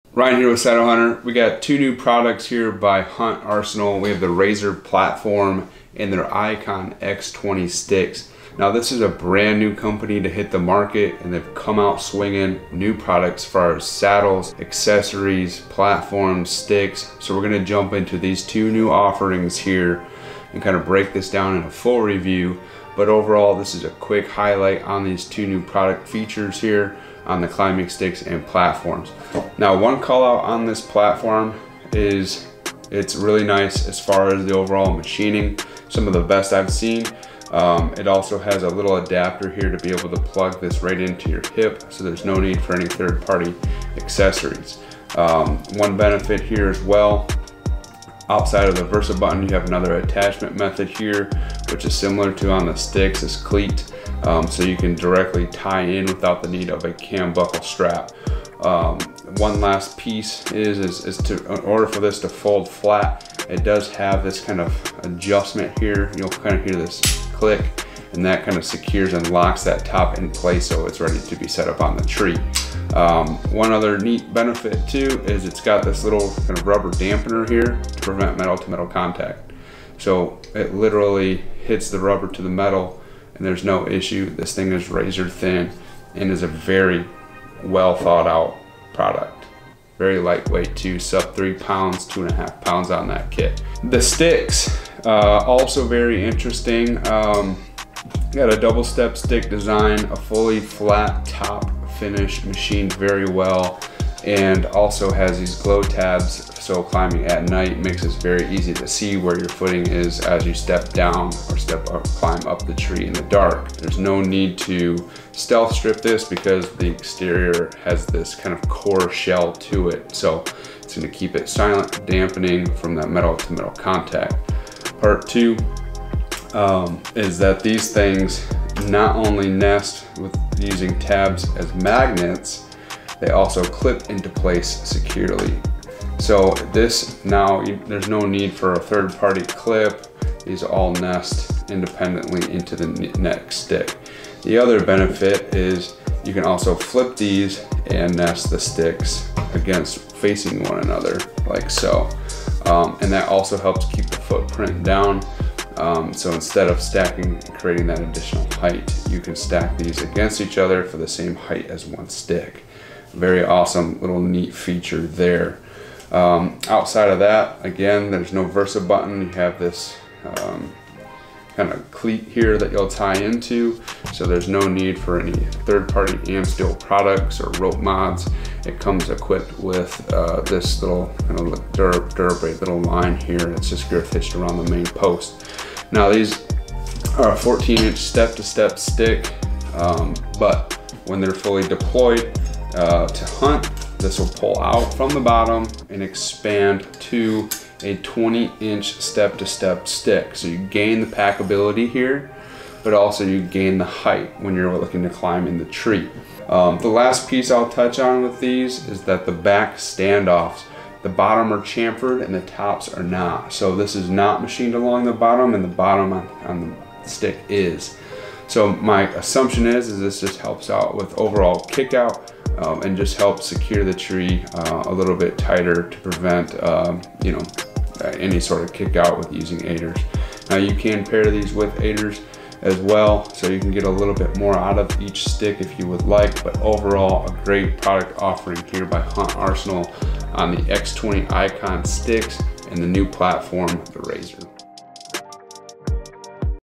Hunt Arsenal RZR and ICON X20 Sticks
Here's the quick skinny on the RZR Platform and X20 Climbing sticks.  The overall quality from Hunt Arsenal is some of the best I've seen.  Check out my initial impressions and thoughts on the new product offerings from Arsenal.